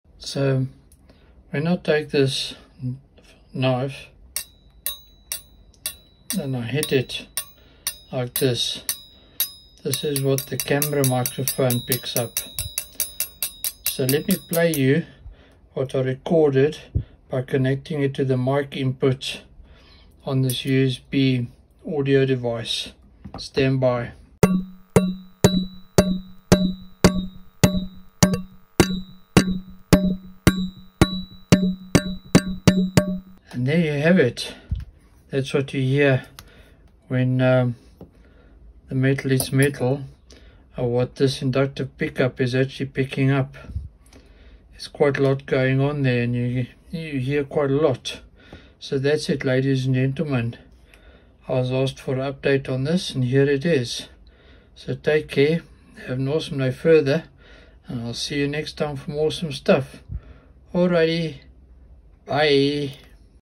Recording the sounds of cutlery being struck directly into pc
Using the inductive pickup stuck to a piece of table cutlery I was able to record amazingly realistic sound of the item vibrating.